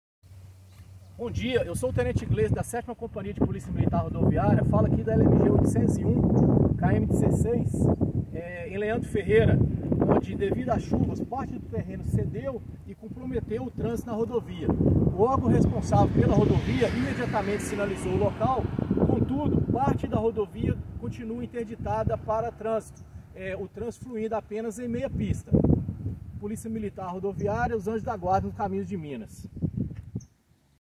Queda de barreira ,Meia pista interditada sentido Leandro Ferreira
👮‍♂️🚧 *Queda de barreira, meia pista interditada*

*Data:* 04/12/2022
*Horário do fato:* 17:40
*Local:* LMG 801 KM 16, Nova Serrana

*Síntese:* Barranco na margem do sentido decrescente cedeu, causando risco para a segurança da via. Meia pista interditada. DER contactado e empresa terceirizada foi acionada e já tomou as providências iniciais com a sinalização do local.